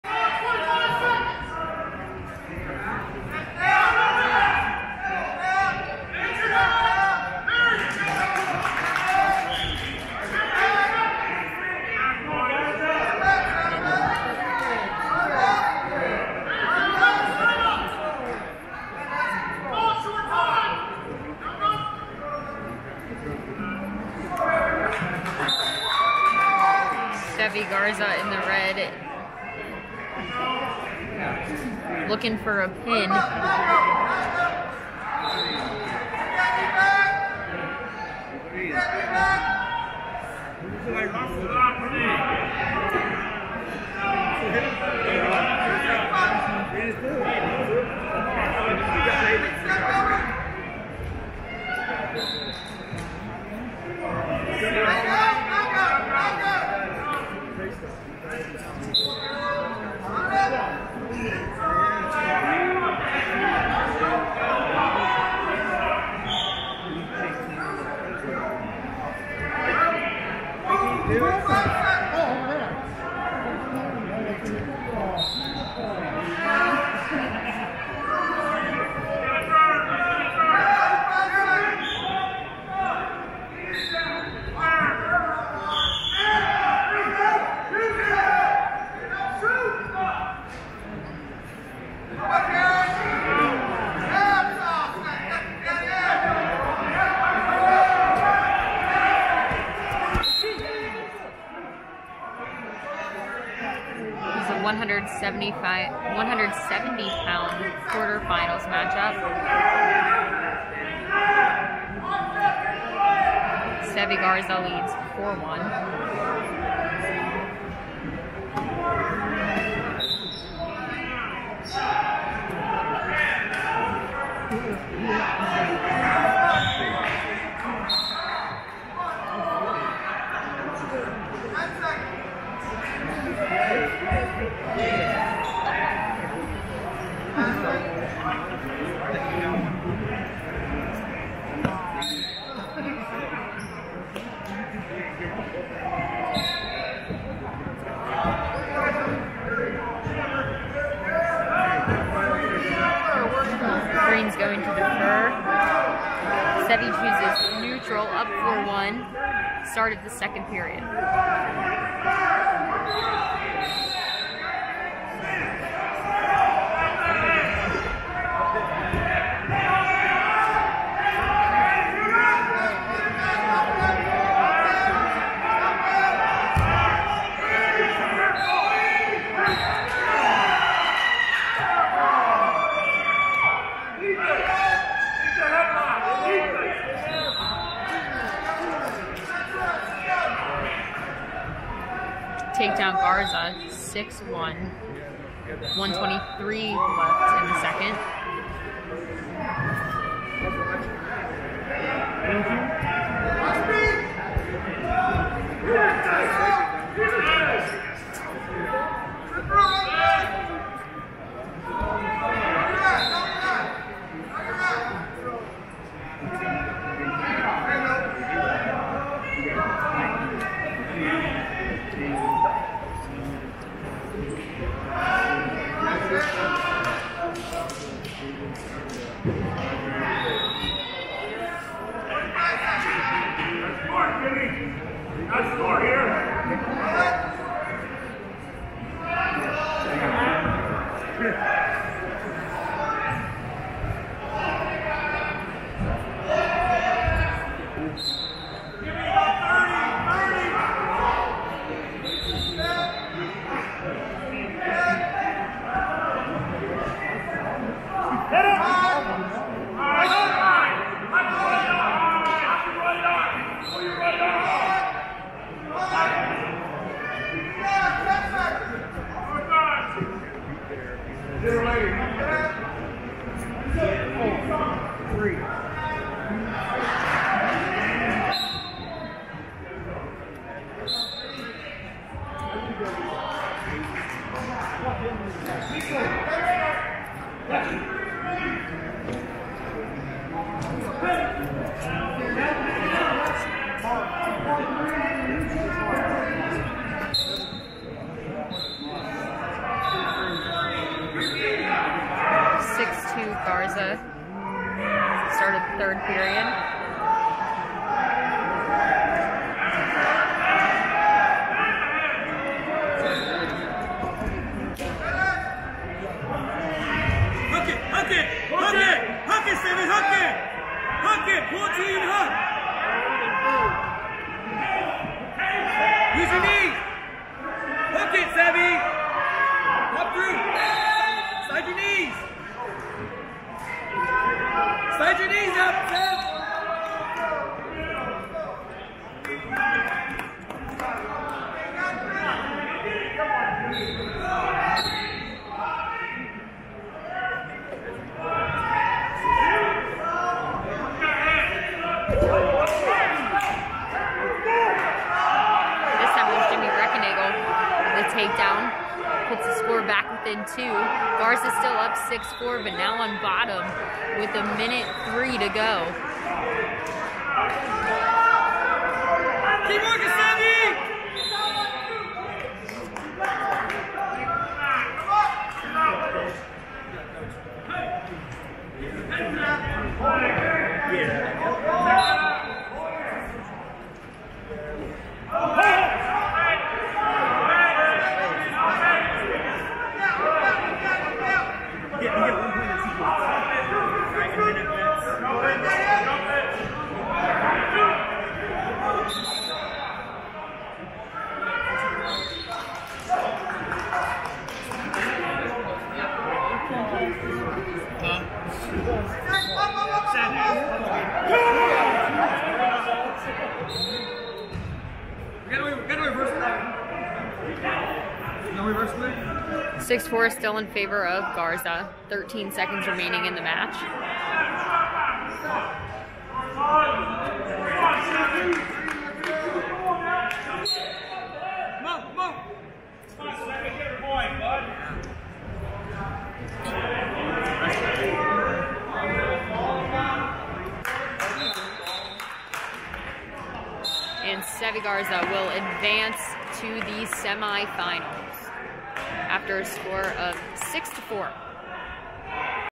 Sevi Garza vs. Jimmy Recknagel: State Quarter Final
Sevi Garza defeats Jimmy Recknagel 6-4 in the 170 pound 2021 state quarterfinal.